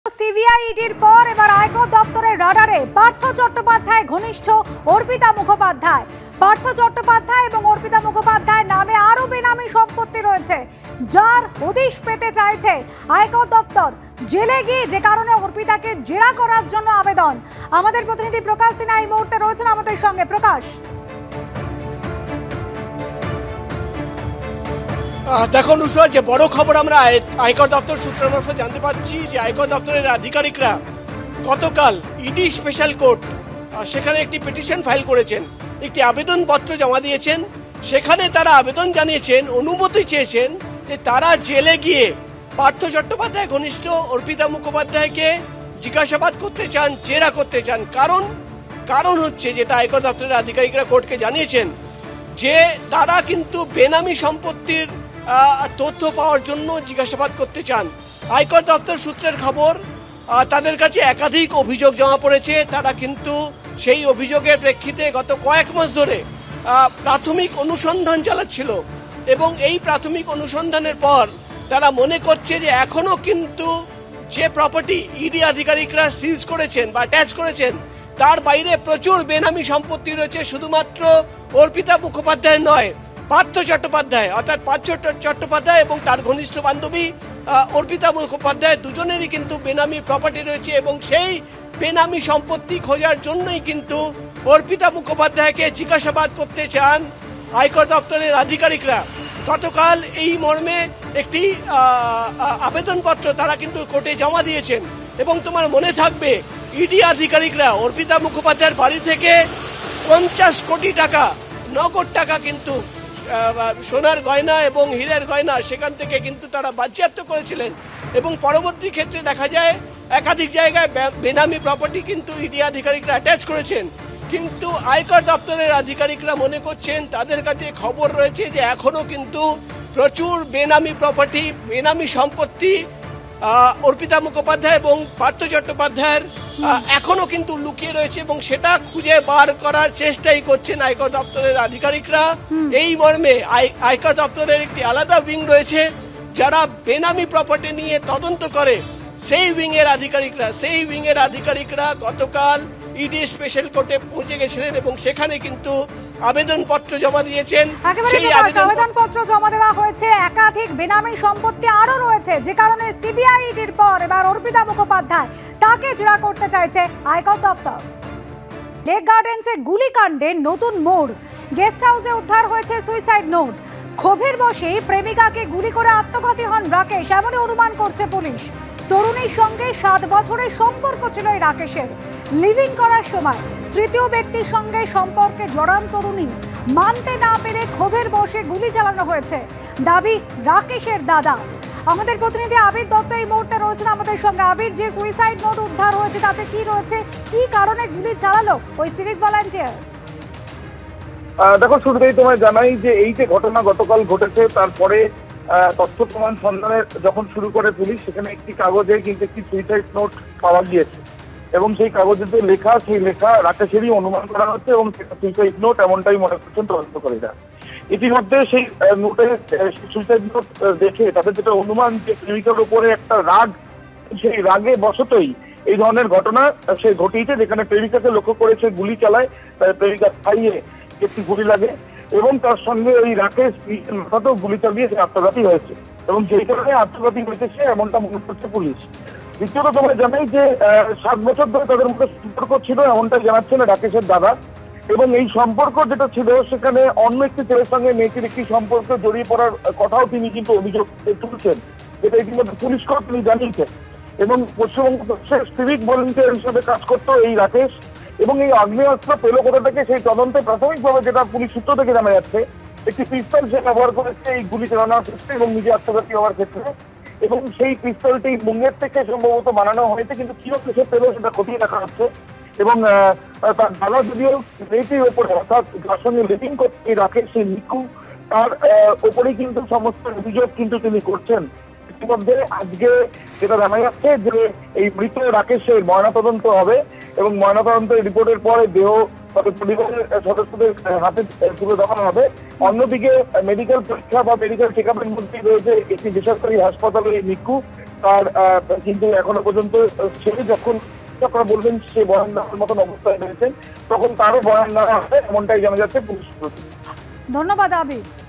Parth Arpita More Assets: পার্থ-অর্পিতার নামে রয়েছে আরও বেনামি সম্পত্তি, জেলে গিয়ে অর্পিতাকে জেরা কর
ABP Ananda Live: সিবিআই, ইডি-র পর এবার আয়কর দফতরের রাডারে পার্থ-ঘনিষ্ঠ অর্পিতা। পার্থ-অর্পিতার নামে রয়েছে আরও বেনামি সম্পত্তি, দাবি আয়কর দফতর সূত্রের। জেলে গিয়ে অর্পিতাকে জেরা করার আবেদন। বেনামি সম্পত্তি খুঁজতেই অর্পিতাকে জেরা প্রয়োজন, আদালতে দাবি আয়কর দফতরের, খবর আয়কর দফতর সূত্রে।